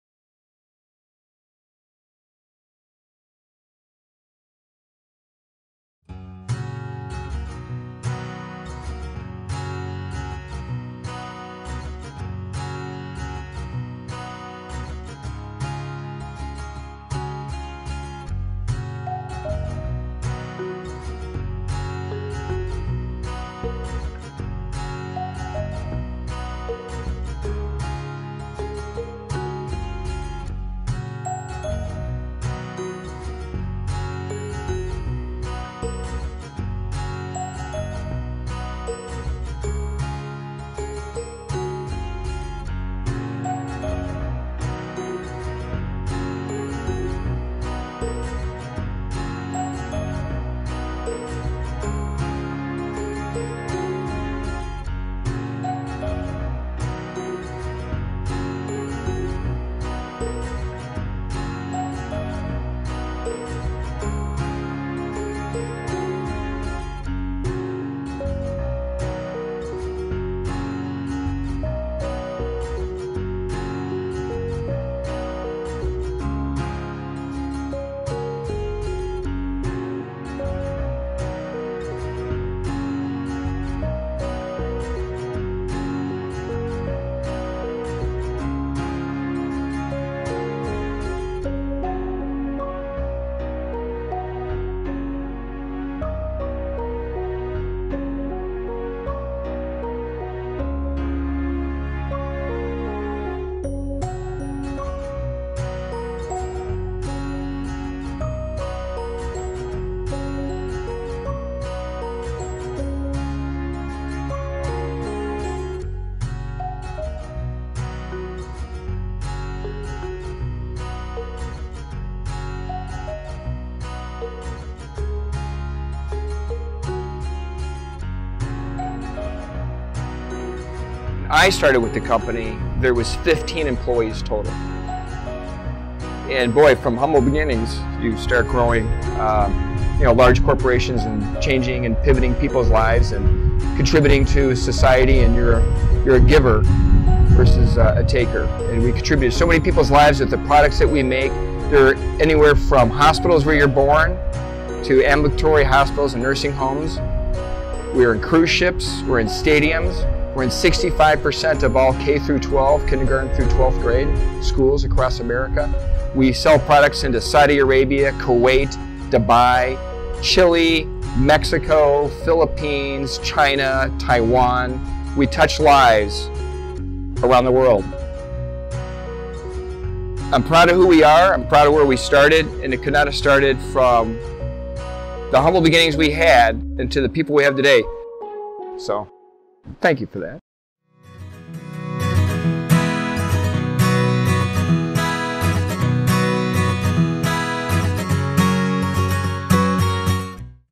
FWE Says "Thank You"
FWE says "Thank You" to employees by celebrating with a cook out to honor all of the hard work and company growth!